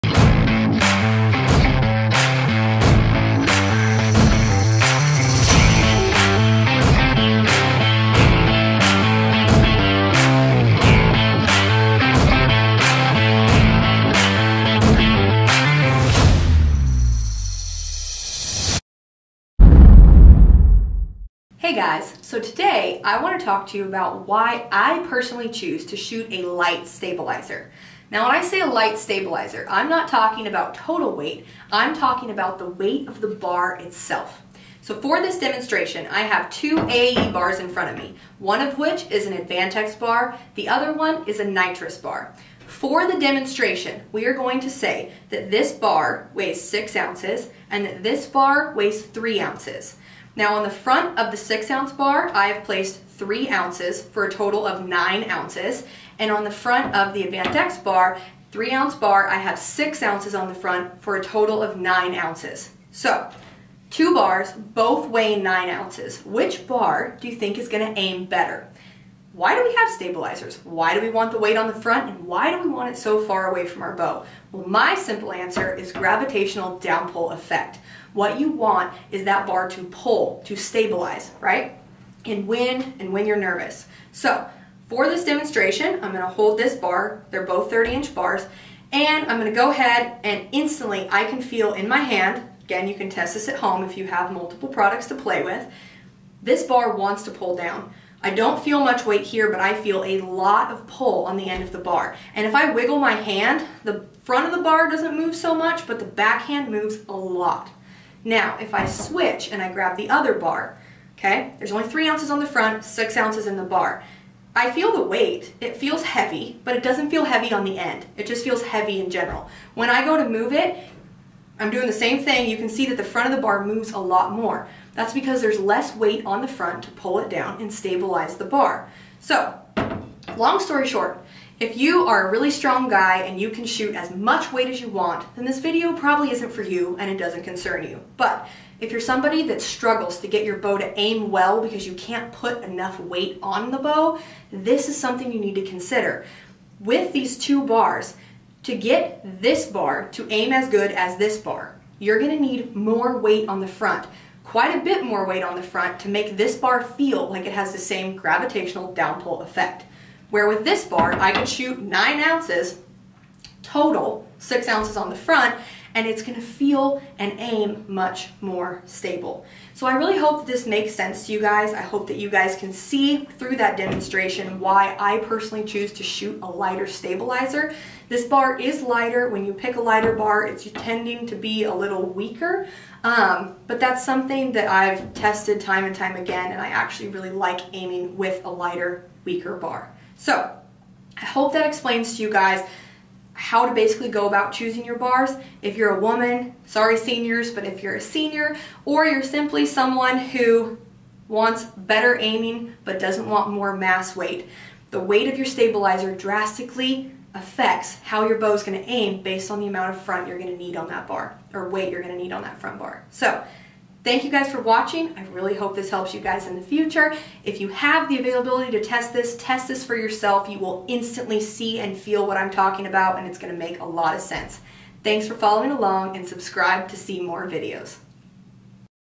Stabilizer Mass Weight: Light vs Heavy
This is incredibly important for anyone that wants to aim better, but can't hold up a ton of mass weight!